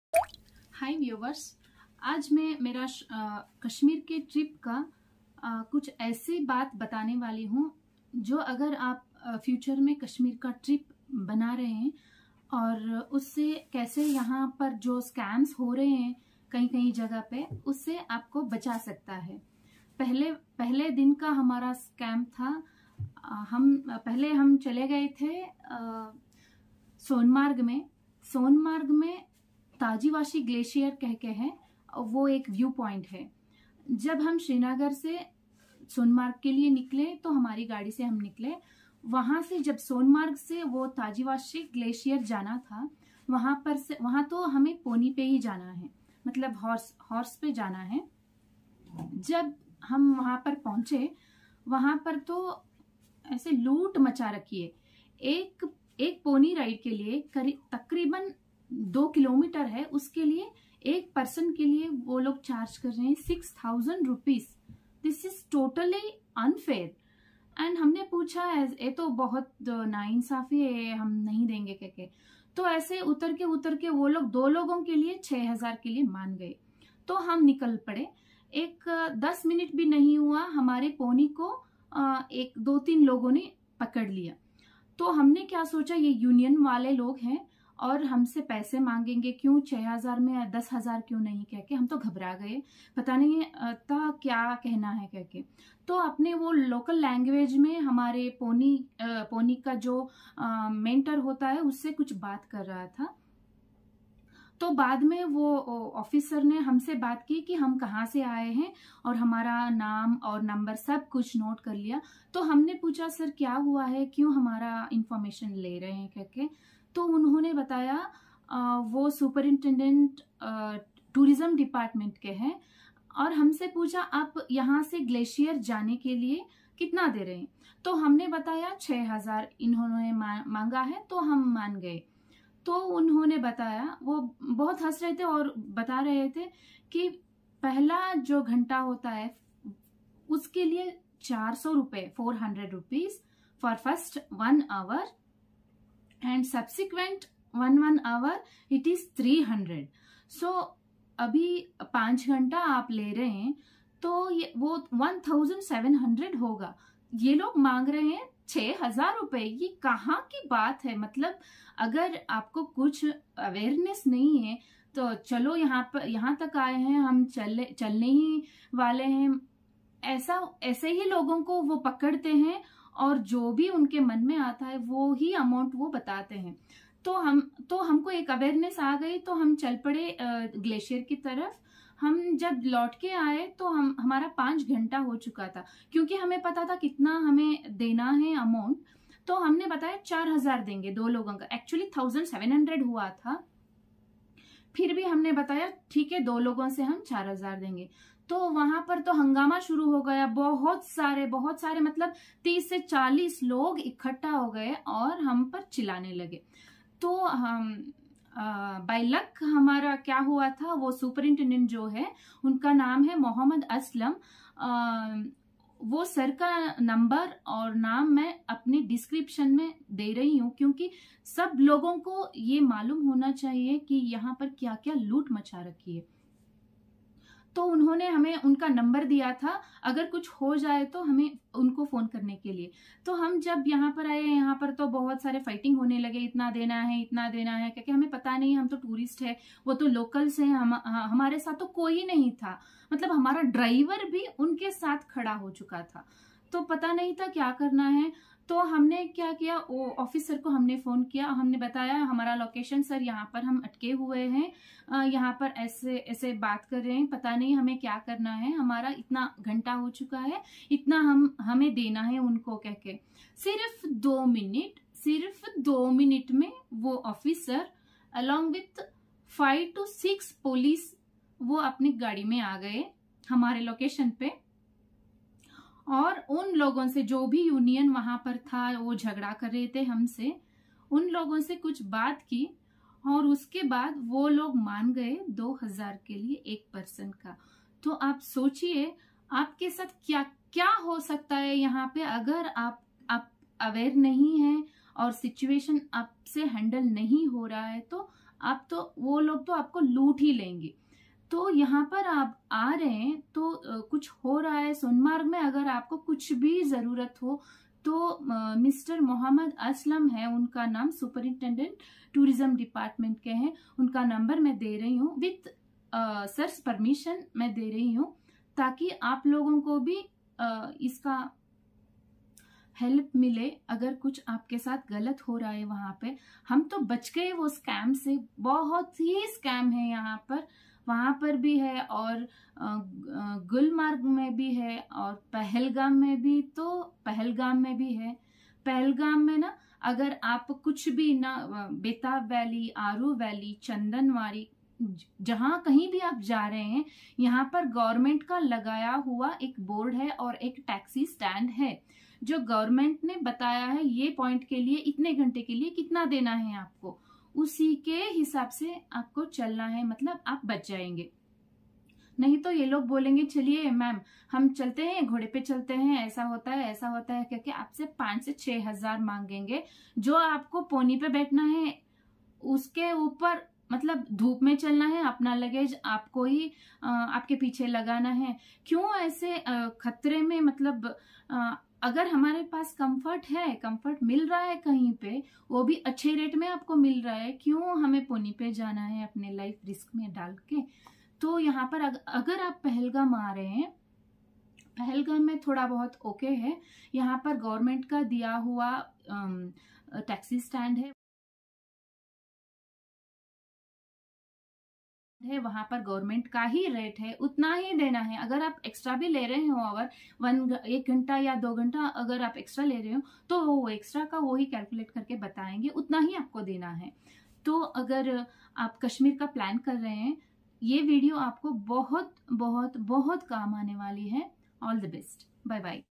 Tourist scam in Sonmarg - Pahelgam - Kashmir - Be smart- Be alert
From Sonmarg Pony stand to Tajiwasi glacier - price -

1st - 1hrs - Rs 400/
Subsequently Rs. 300/- per hrs
Boots - 1 pair - Rs. 100/-

We took 5 hrs for the sightseeing.
As per the prescribed rate - Rs. 1600/- + Rs. 100/- boots = Rs. 1700/-

Mr. Mohd. Aslam -  Tourist Officer , Tourism Department -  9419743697

Latest rates (As on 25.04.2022)

Rs.495/- for 1st hour and 330/- for subsequent hours and from pony stand to thajiwas is Rs.715/- to and fro and Rs. 220/- waiting charge per hour  from thajiwas to Glacier it is Rs. 200/- more and waiting charge Rs. 220/ per hour.